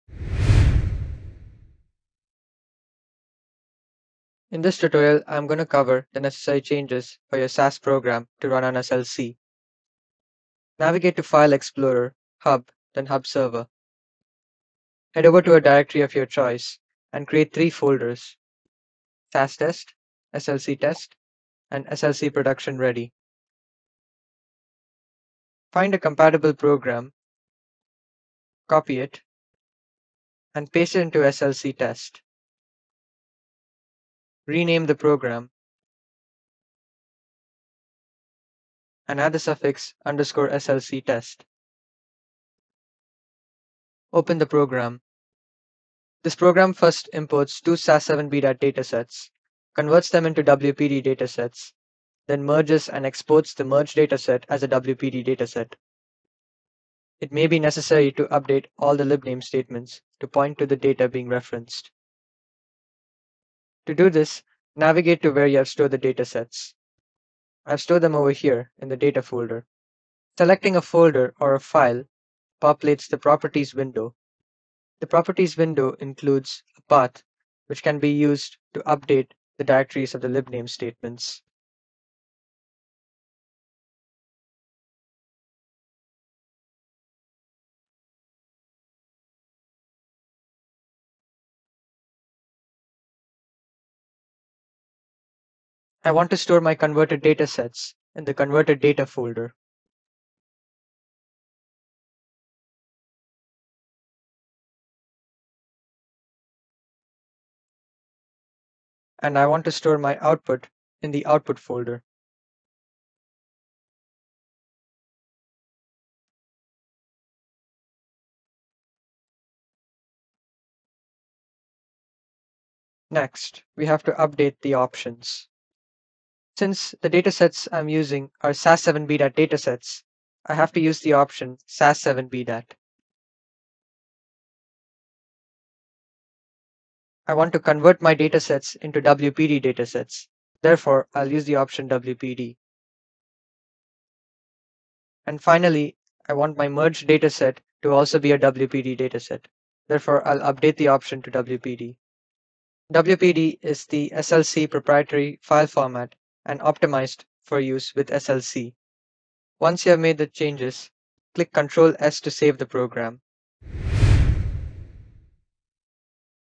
Prepare SAS Language Programs to Run on Altair SLC Part 1
This is part 1 of a tutorial on making the changes necessary to run SAS programs on SLC.

This program first imports 2 SAS7BDAT datasets, converts them into WPD datasets, then merges them and exports the merged dataset as a WPD dataset. It may be necessary to update all libname statements to point to the data being referenced. To do this, navigate to where you have stored the datasets. I have stored them in the data folder. Clicking on any dataset in this location populates the properties window. The properties window includes the item path, and this can be copied from here and then pasted into the appropriate libname statement. WPD is the SLC proprietary file format and optimized for use with SLC. To save files as WPD, simply update the libname statement library format from SAS7BDAT to WPD. The program files are saved to WPD format and all libname statements are updated accordingly.